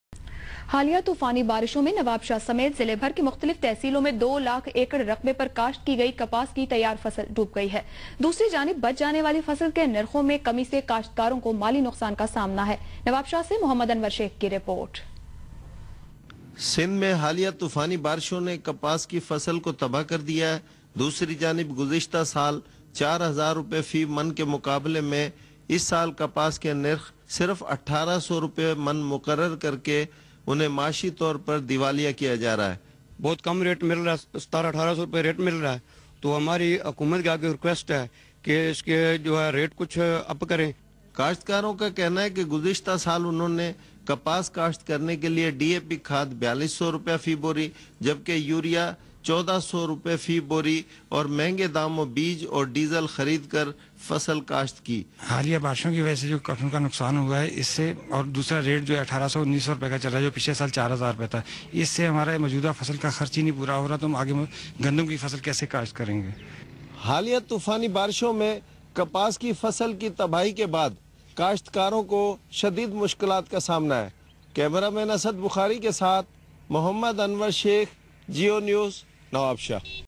Geo Anwar Shaikh Nawabshah Rain. Cotton Damaged Report.
Asad.03123220061